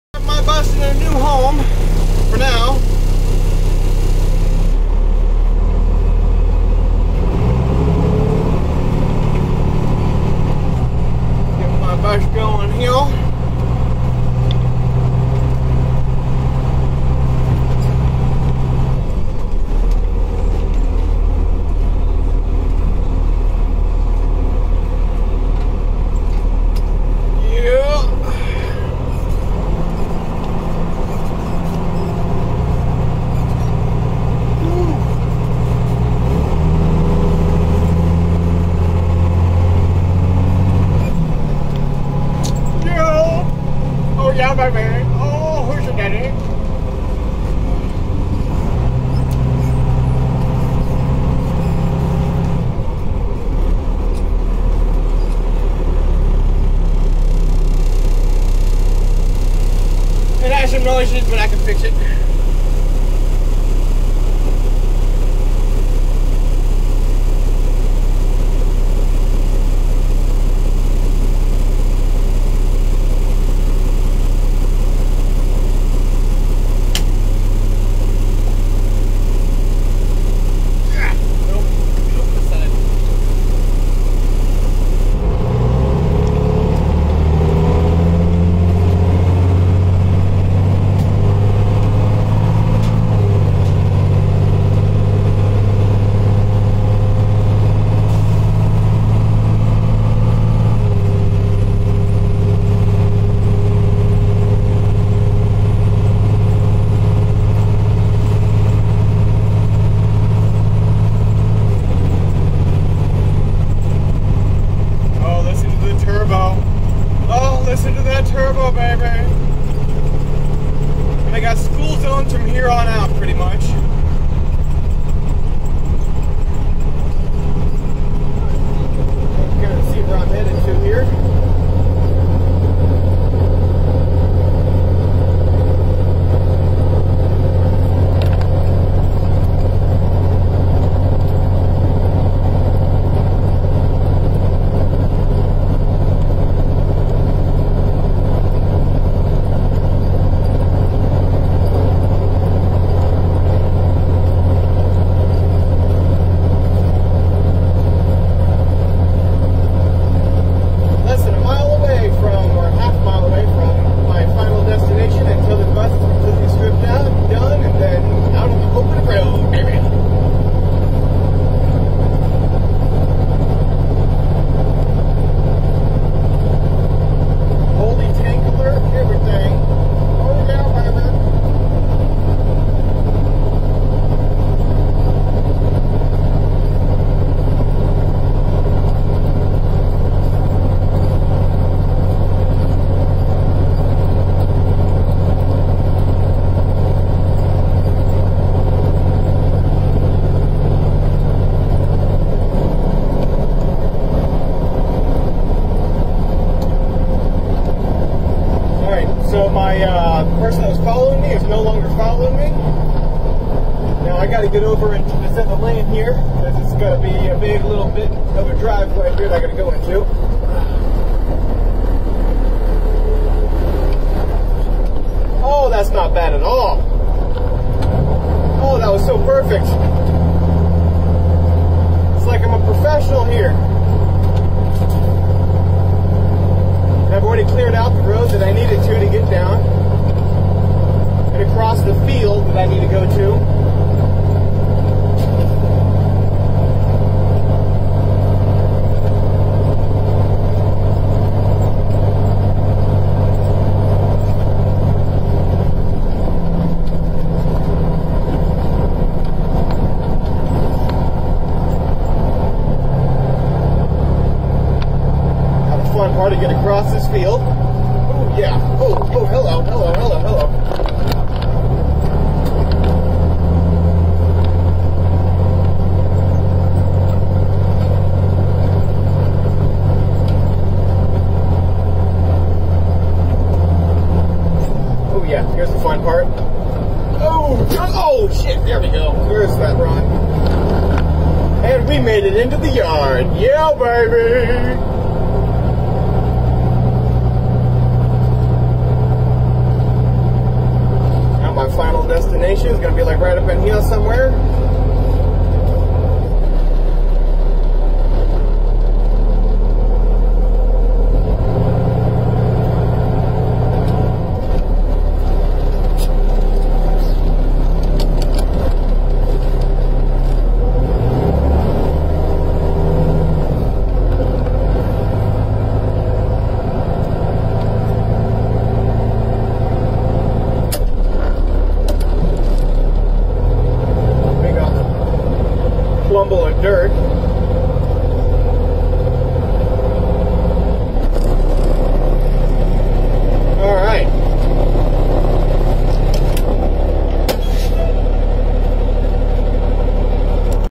1st skoolie video getting the bus home
Bought my bus a week before and had to clear the yard for her arrival... About 9 days later I was able to get her home to the conversion site... I bought a 1995 Bluebird TC2000 FE
NOTE : Where the bus was picked up from was 10 miles away from my construction site reason why the video is so short.